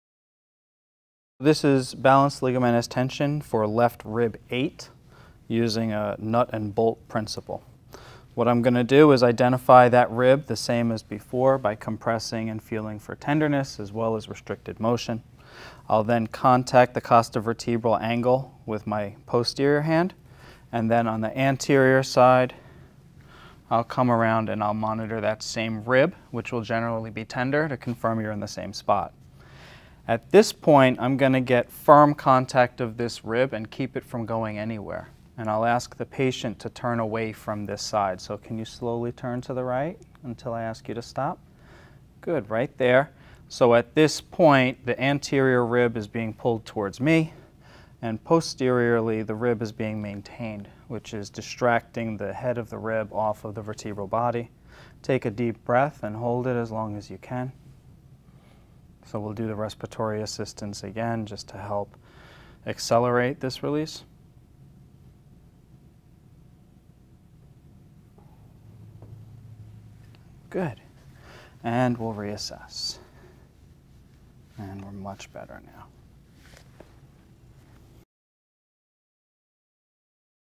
Balanced Ligamentous Tension for Left Rib 8 Using Nut and Bolt Principle
2019 OMT Demonstration Videos - 6 of 9
Rowan University School of Osteopathic Medicine

Balanced Ligamentous Tension for Left Rib 8 Using Nut and Bolt Principle - OMT Techniques

Jonathan Torres, D.O.
Family Medicine, NMI Osteopathic Manipulative Medicine
One Medical Center Drive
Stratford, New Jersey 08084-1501